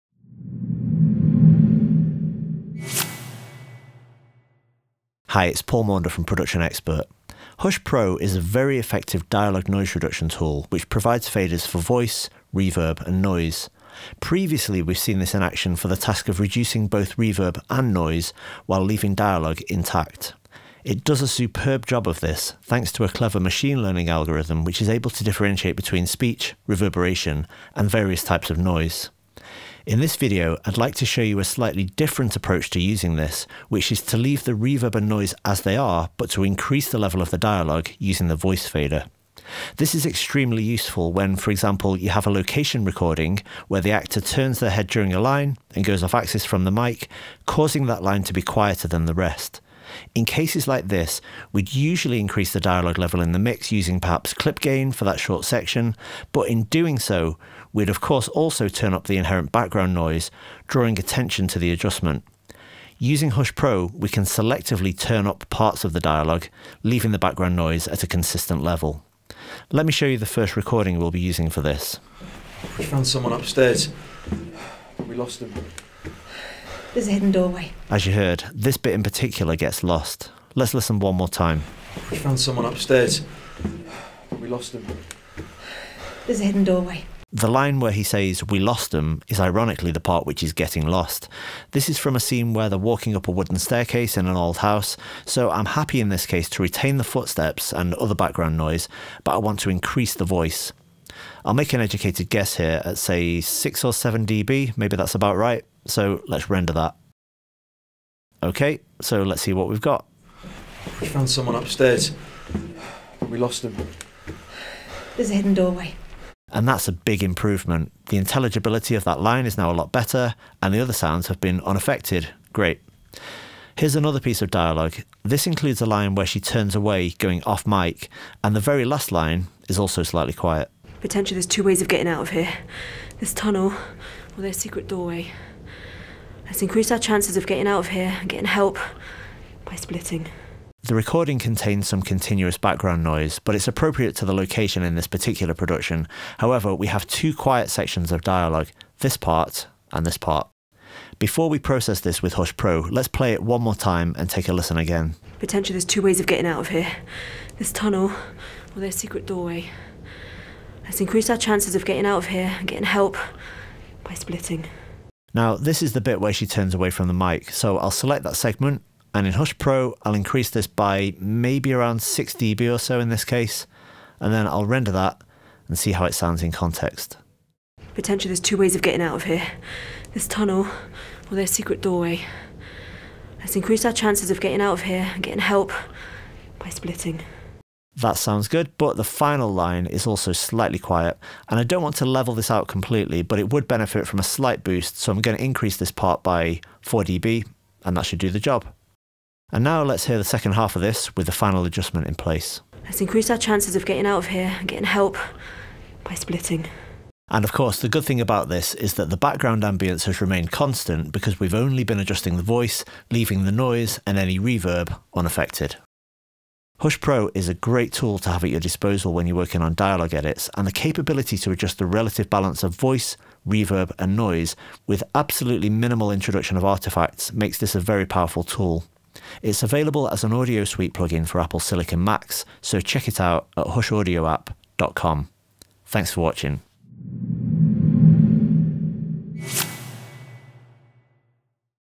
Rebalancing Dialogue With Hush Pro
Thanks to its powerful AI-driven noise and reverb reduction, Hush Pro has quickly become a go-to tool for cleaning up dialogue. But beyond the basics, it also offers smart ways to enhance dialogue without affecting background ambience. In this video, Paul Maunder shows you how.